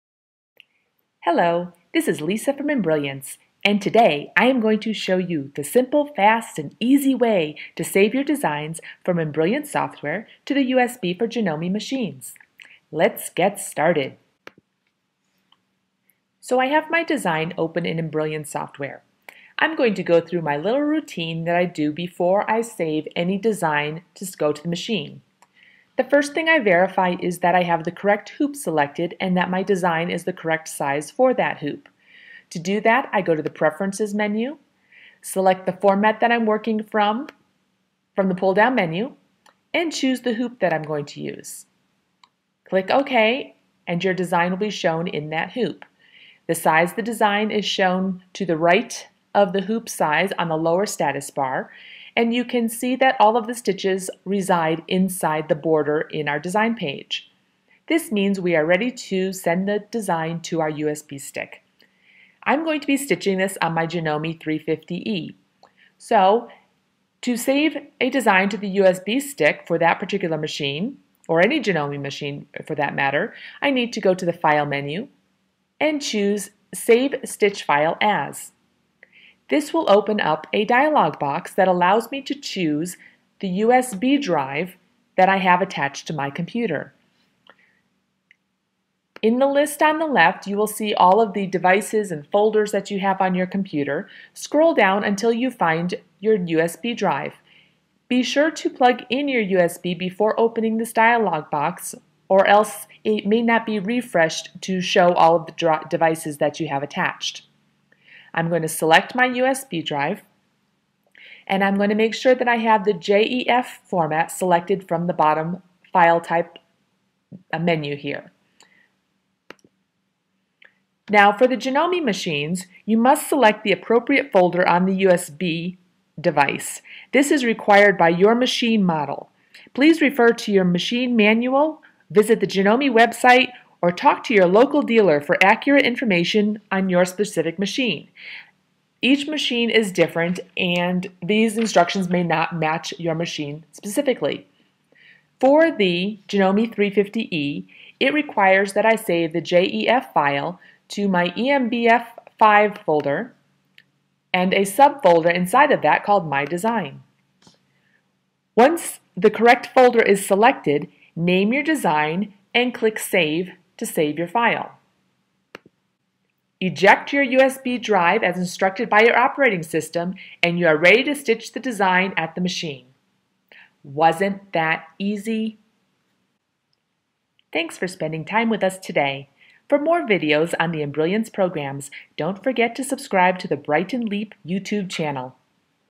Saving Designs to the Janome machine
Save designs to your Janome machine the simple, fast, & easy way using Embrilliance software.